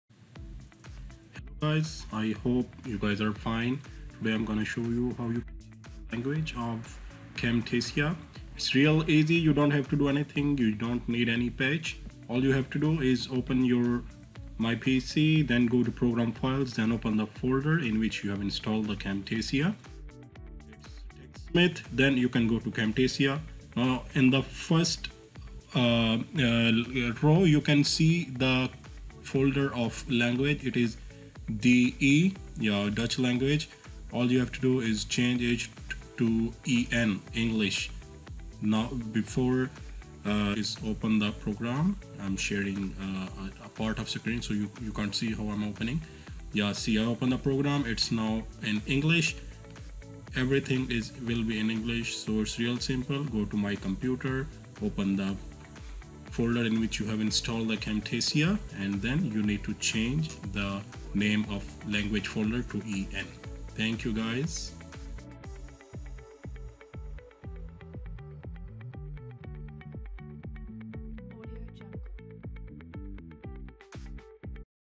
Change language of Camtasia to English
It's really easy to change the language of Camtasia.

All you have to do is to change the name of folder "de-DE"  to  "en-EN"